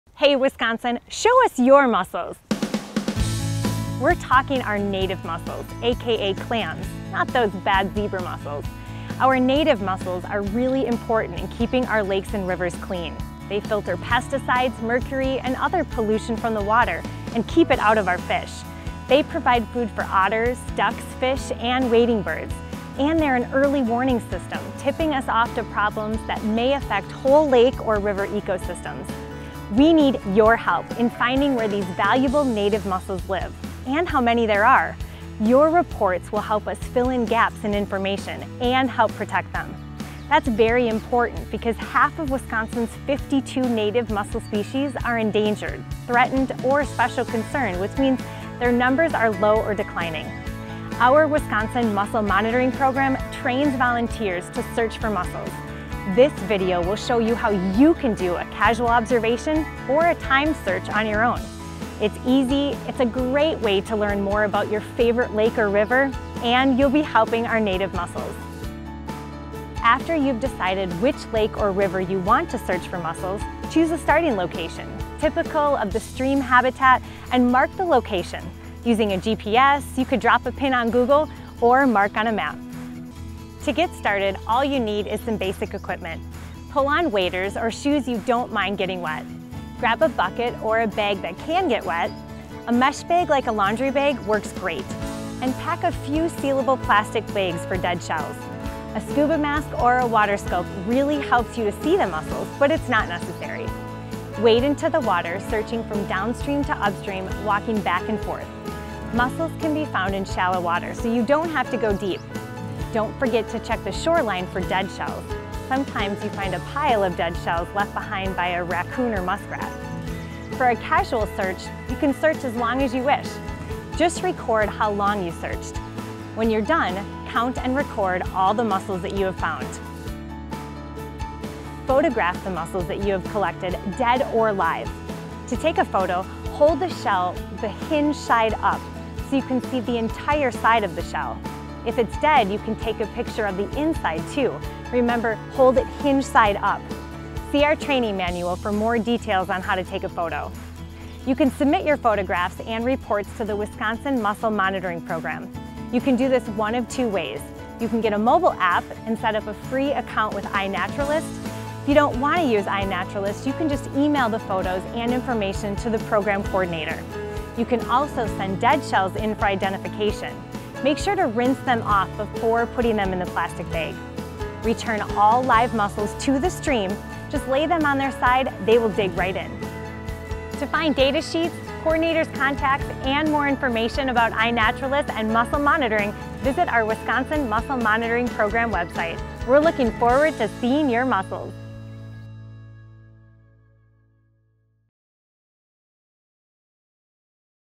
How to search for and report Wisconsin’s native freshwater mussels
Wisconsin’s freshwater mussels are important for clean lakes and rivers. Follow the easy steps in this video to help us search shorelines and shallow water for native mussels and to report what you find to the Wisconsin Mussel Monitoring Program.